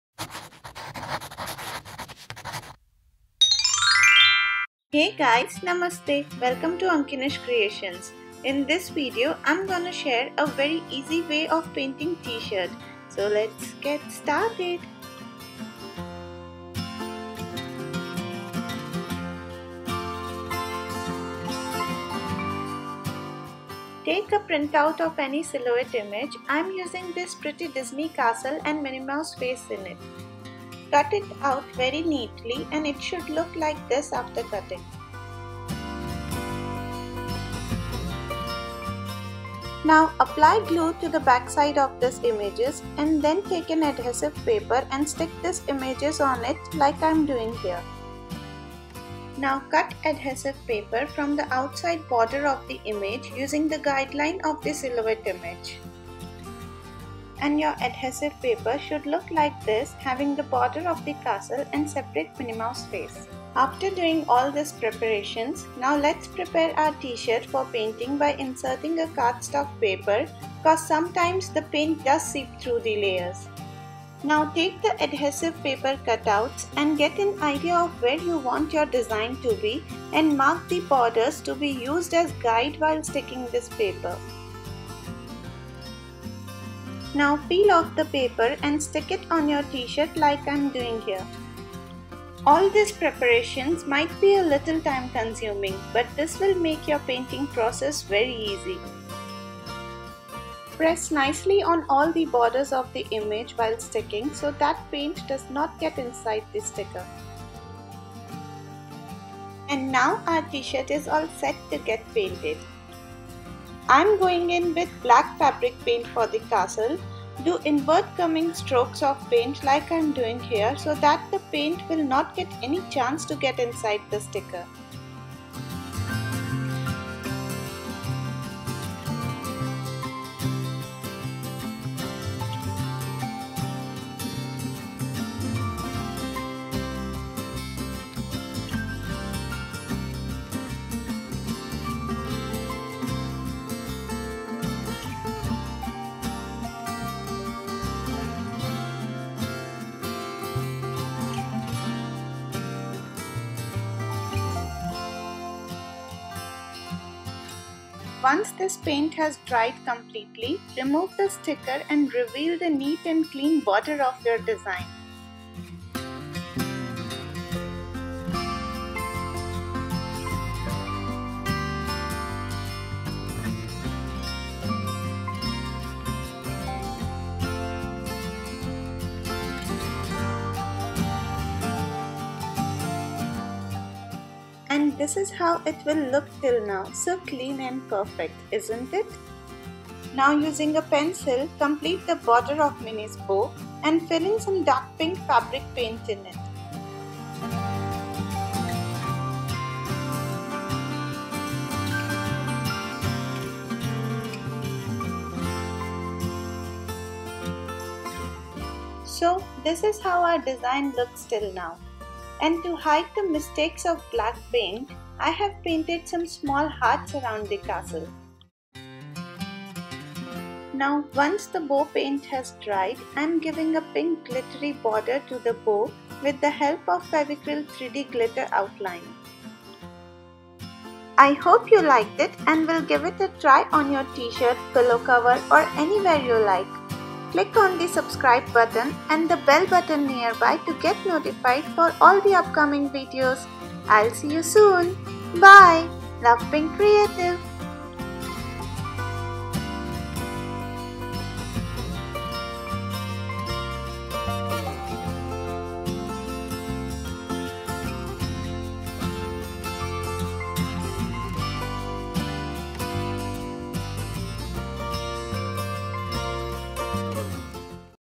DIY: Easy T-shirt painting!! Disney castle silhouette print at home, very easy!!
Hi Guys,
In this video, I am sharing a very easy technique of painting a silhouette image on T-shirt and make your T-shirt unique. This can be a magical gift for any disney fan :) You can try this DIY on T-shirts, Pillow covers etc and add Disney magic to your life.
I hope you liked it and will give it a try!!

Love being creative!!

See you soon :)